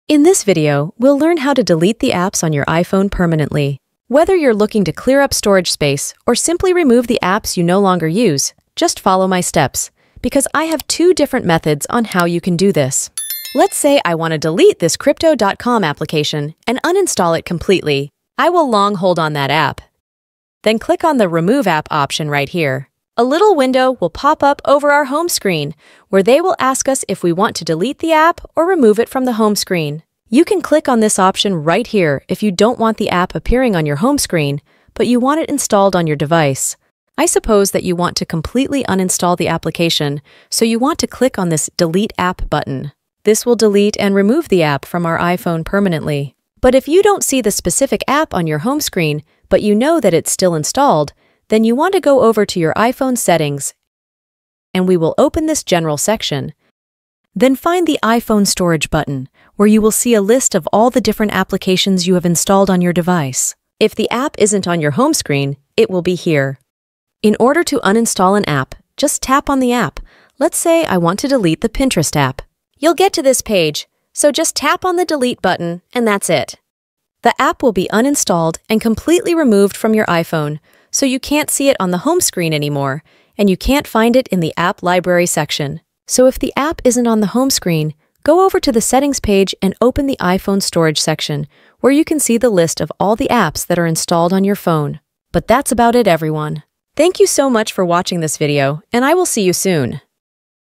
How to Delete Apps on iPhone Permanently (2024)
In this comprehensive tutorial, learn how to permanently delete apps on your iPhone in just a few simple steps. Whether you're looking to free up storage space or declutter your home screen, just follow my steps to learn how it's done.